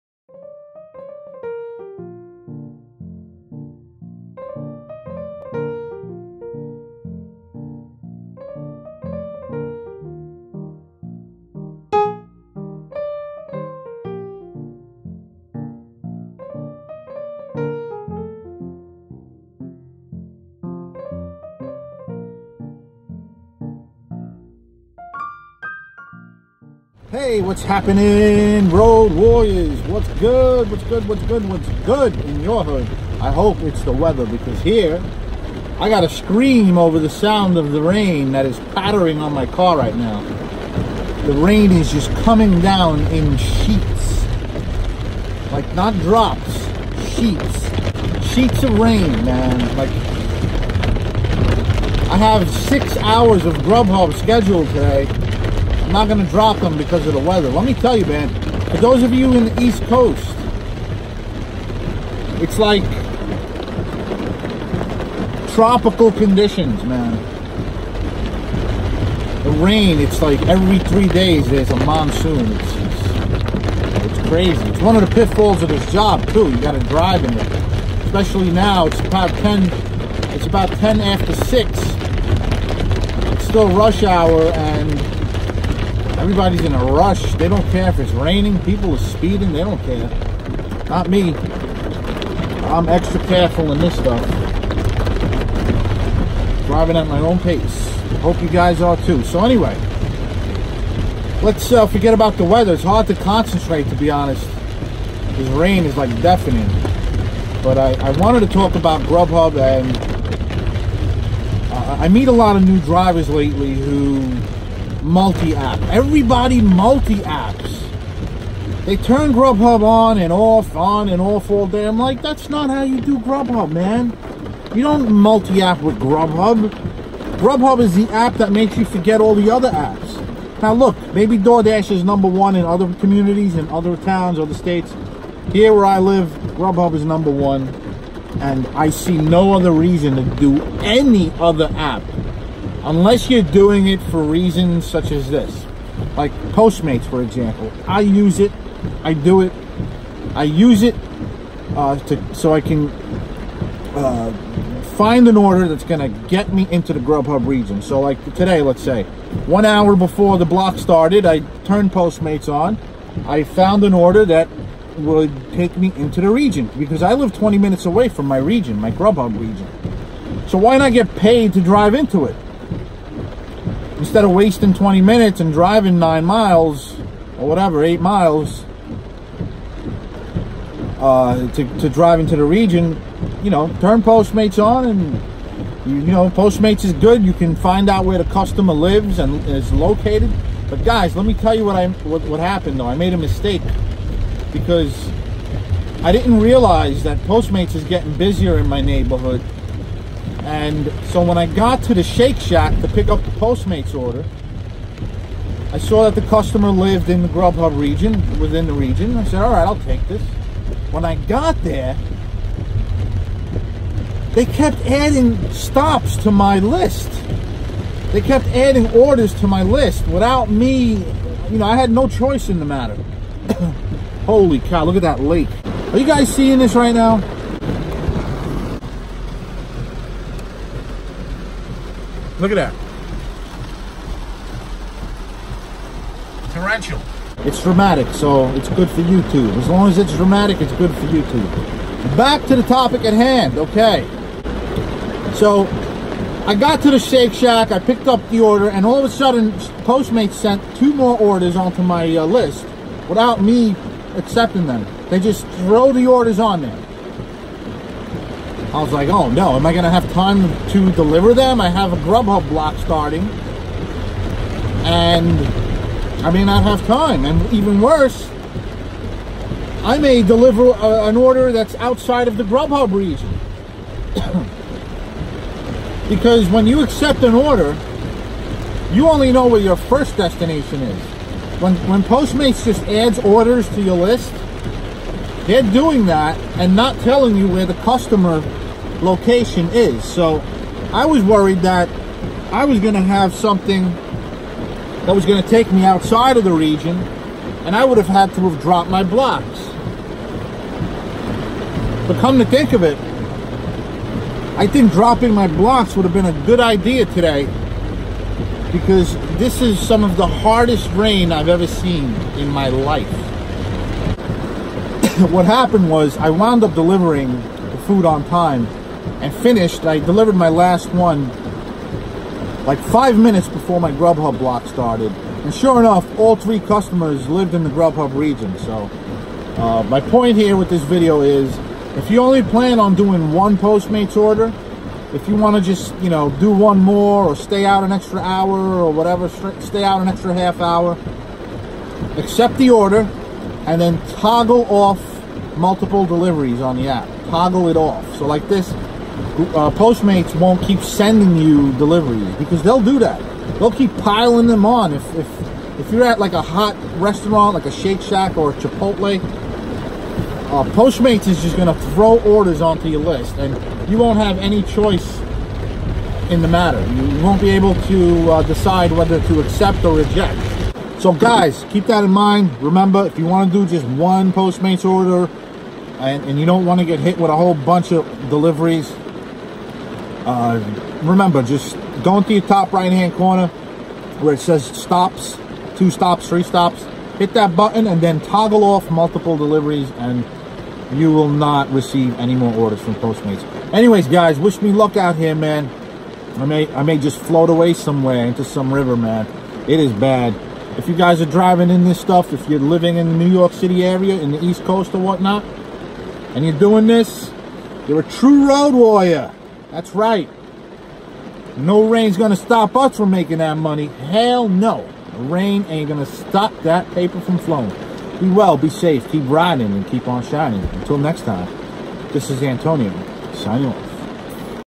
GRUBHUB delivering in a rainstorm and a tip on Postmates
Ride along with me as I avoid puddles, rivers, and streams so I can get people their food! And a word of advice for Postmates.